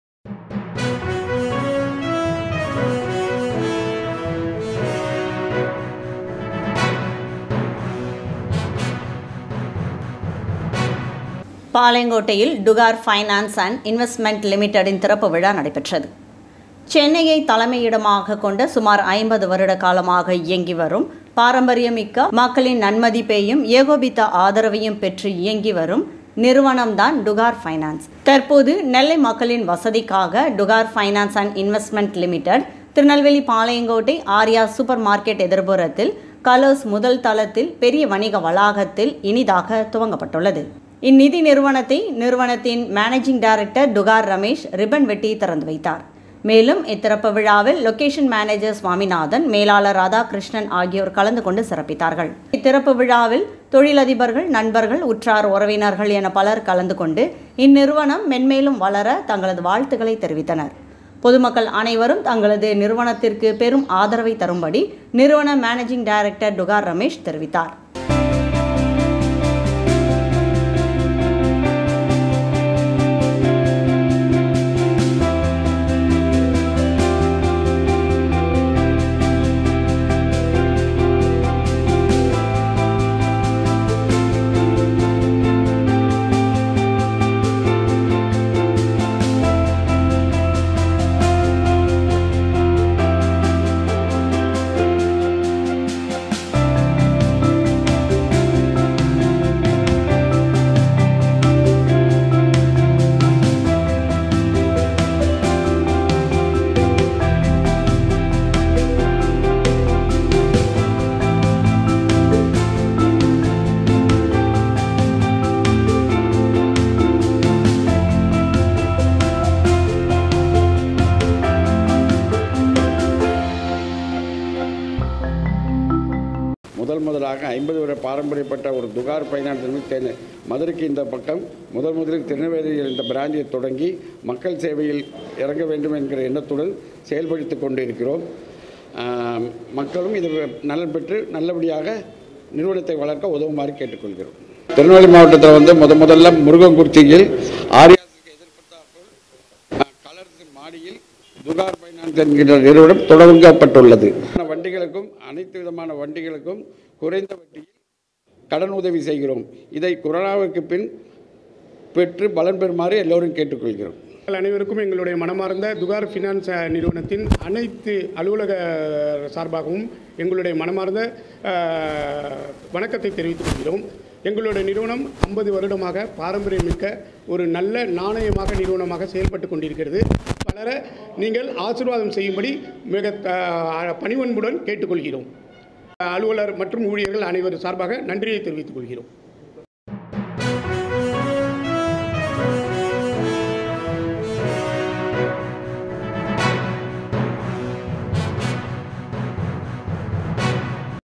டுகார் பைனான்ஸ் அண்ட் இன்வெஸ்ட்மெண்ட் லிமிடெட் திறப்பு விழா நடைபெற்றது | TAMIL | MAYURITV
சென்னையை தலைமை இடமாக கொண்டசுமார் 50 வருட காலமாக இயங்கிவரும் பாரம்பரியமிக்க மக்களின் நம்பிக்கையும் ஏகோபித்த ஆதரவையும் பெற்று இயங்கிவரும் நிறுவனம் (dugar) டுகார் பைனான்ஸ் அண்ட் இன்வெஸ்ட்மெண்ட் லிமிடெட் தற்போது நெல்லையில் மக்களின் வசதிக்காக திருநெல்வேலி பாளையங்கோட்டை ஆர்யாஸ் சூப்பர் மார்க்கெட் எதிர்புறத்தில் கலர்ஸ் முதல் தளத்தில் பெரிய வணிக வளாகத்தில் 16 /09 /2021 /முதல் இனிதே துவங்கப்பட்டுள்ளது இந்நிதி நிறுவனத்தை நிறுவனத்தின் மேனேஜிங் டைரக்டர் டுகார் ரமேஷ் ரிப்பன் பட்டி திறந்து வைத்தார் மேலும் இத்திறப்பு விழாவில் லொகேஷன் மேனேஜர் சுவாமிநாதன், மேலாளர் ராதாகிருஷ்ணன்,ஆகியோரும் கலந்து கொண்டு சிறப்பித்தனர் தொடர்ந்து இத்திறப்பு விழாவில் தொழிலதிபர்கள் நண்பர்கள் உற்றார் உறவினர்கள் என பலர் கலந்துகொண்டு இந்நிறுவனம் மென்மேலும் வளர தங்களது வாழ்த்துக்களை தெரிவித்தனர் பொதுமக்கள் அனைவரும் தங்களது நிறுவனத்திற்கு பெரும் ஆதரவை தரும்படி நிறுவன  மேனேஜிங் டைரக்டர் டுகார் ரமேஷ் தெரிவித்தார்